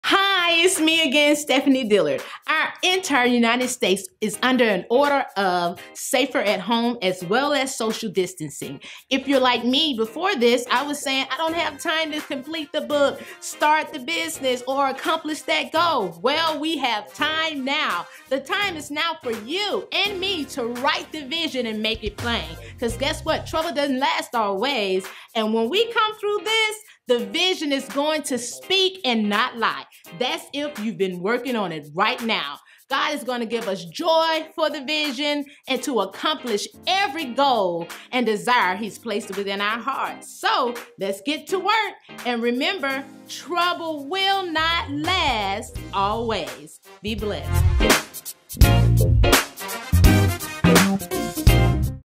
The Time Is Now | Lady Stephanie Dillard
It's a great idea to use this time to focus on our God-given hopes, dreams, aspirations, visions and goals ! However if you are not working on a business plan, writing a book, or going back to school at this time of quarantine, THAT'S OK! COMING OUT OF THIS PANDEMIC A BETTER PERSON, BEING MORE LOVING, KIND, UNDERSTANDING AND LONGSUFFERING IS A GOAL WORTHY TO ASPIRE TO AND TO BE COMMENDED! Move at your own pace, there's no competition! The only competition we should have is becoming better than who we were before this pandemic occurred!